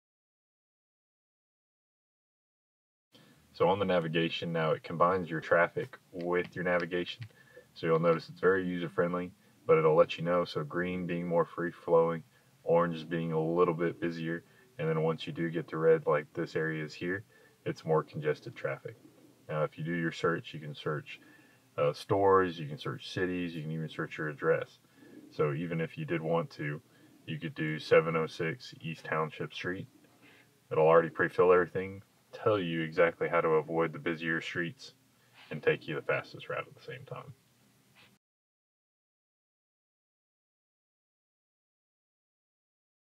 GMC Navigation and Traffic App
This is a walkthrough for the new GMC Navigation app that now includes Real-Time Traffic status.

Superior Automotive Group is here to help you experience the most of your new or new-to-you GMC Sierra. In this video we show you the updated navigation application (on available models) that includes traffic updates and recalculating based on traffic conditions. With easy address and point-of-interest entry, getting to your destination is easier than ever.

If you find yourself with additional questions or want to see more from us